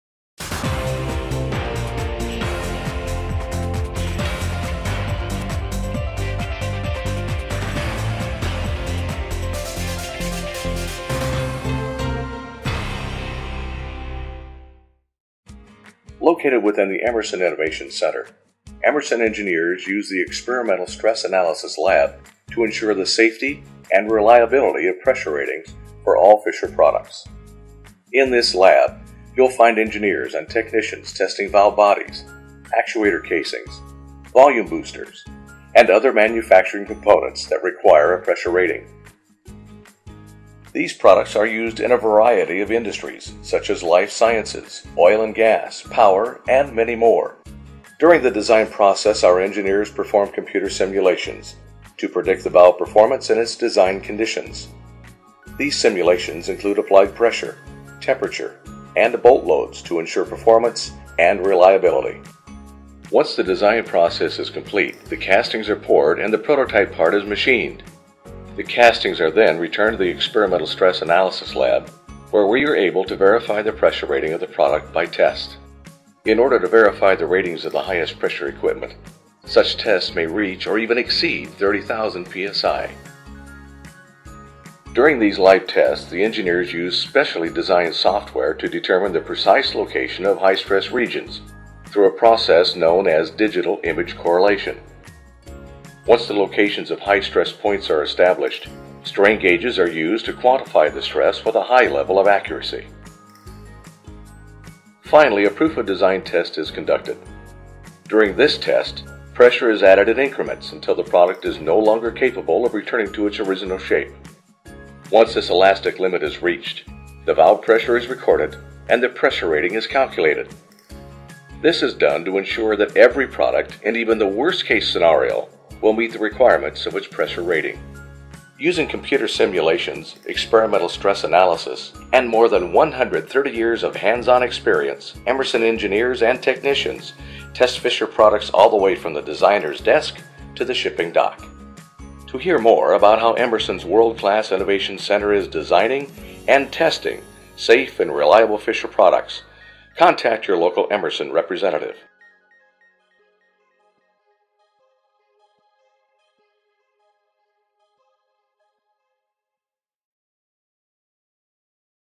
Experimental Stress Analysis Lab in the Emerson Innovation Center
Emerson’s Experimental Stress Analysis Lab in the Emerson Innovation Center is used to verify the accuracy of pressure ratings for Fisher products.